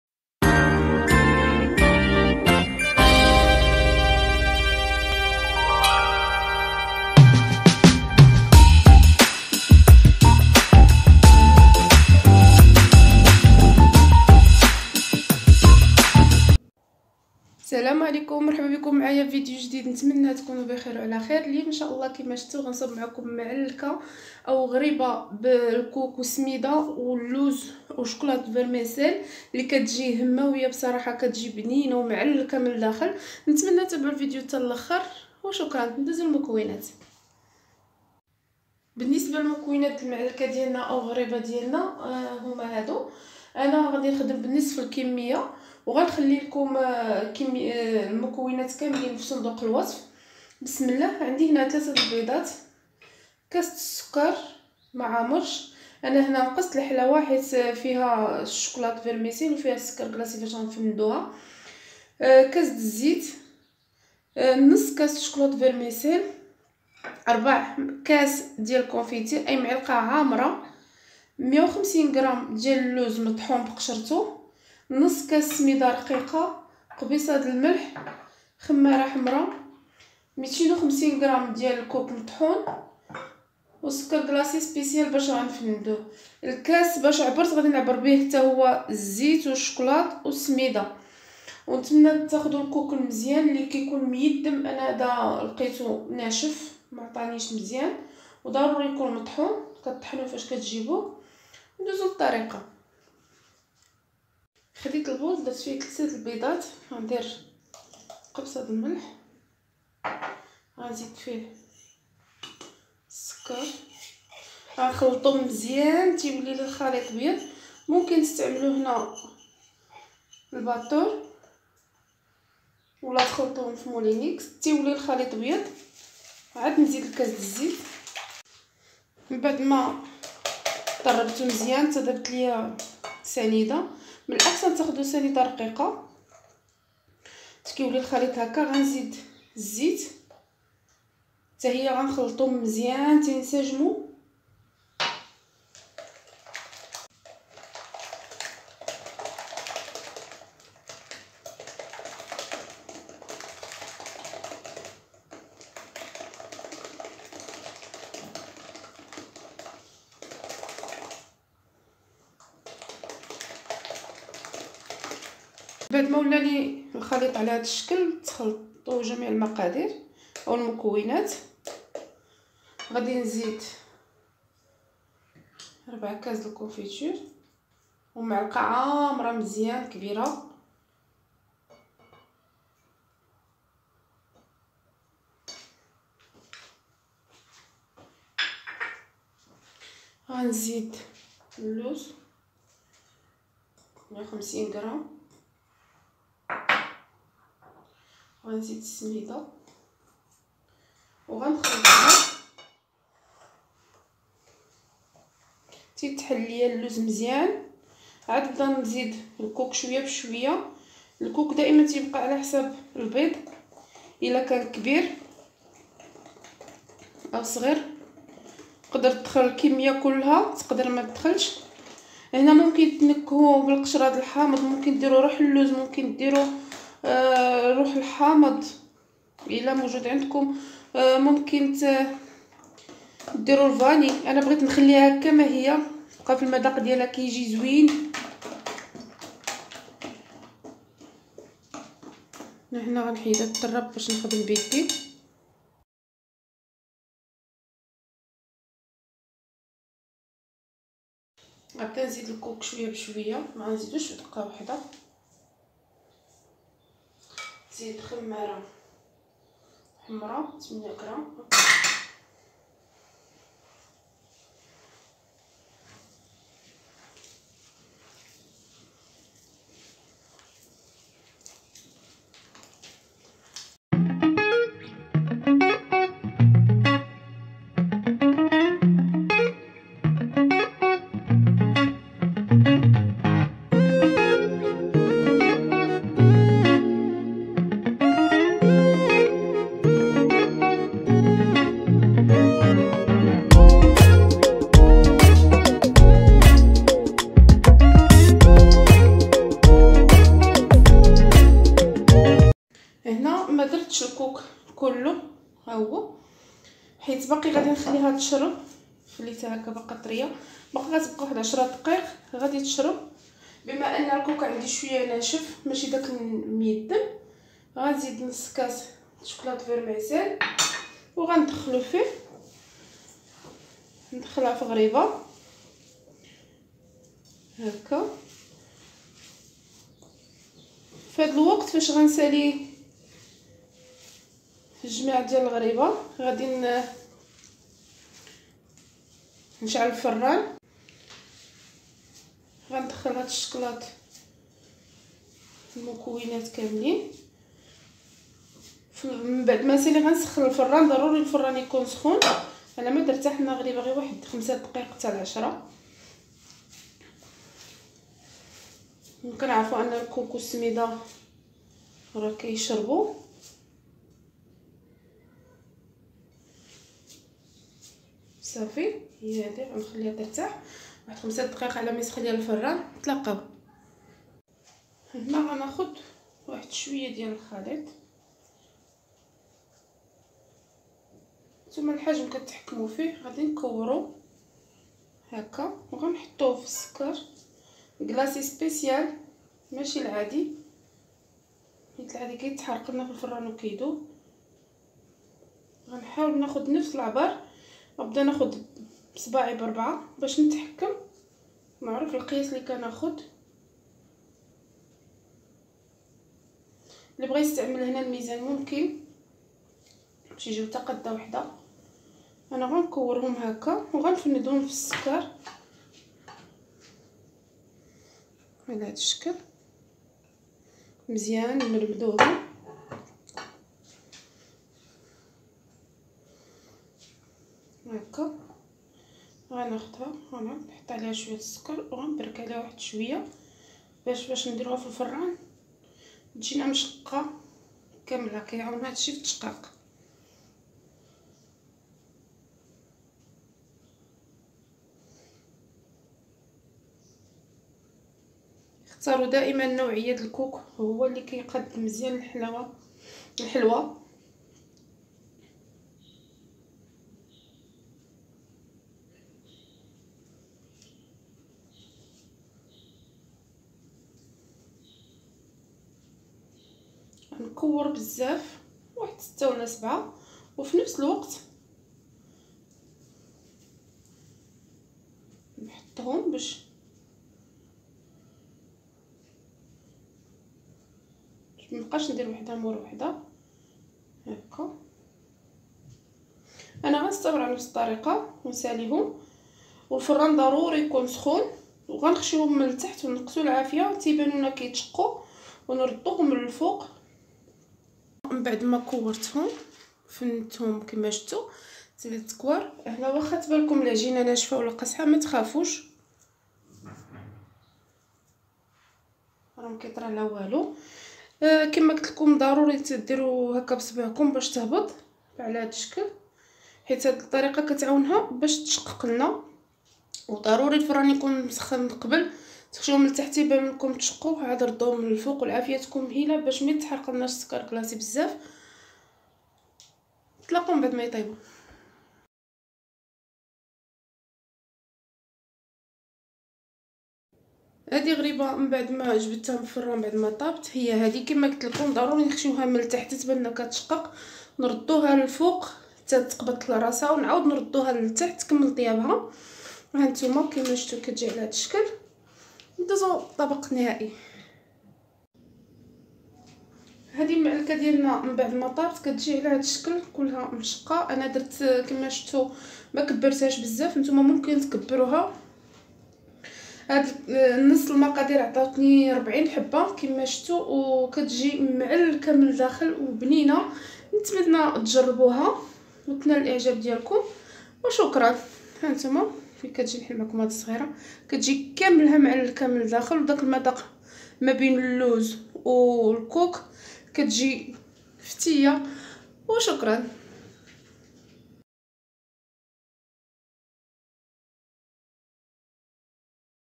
غريبة باللوز وسميدة والكوك وشكلاط فرميسال روعة فالمداق👌👌
💖لا اله الا الله وحده لا شريك له له الملك وله الحمد وهو على كل شيء قدير 💖

القادير👇👇👇👇

6بيضات
2كوؤس زيت
2كؤوس سكر حبيبات:  حلوة دائما حسب الاختيار
كأس شكلاط فرميسال
نصف كاس كونفيتير ( مربى) خاص بالحلويات
 2خمارات حمر(بيكن باودر)
رشة ملح
كاس سميدة رقيقة
نصف كيلو كوك مطحون
250gاللوز مطحون

بنسبة التزين
سكر كلاصي  سبيسيال

💖اذا اعجبكم الفيديو لا تنسواالاشتراك فضلا منكم وليس بامر 💖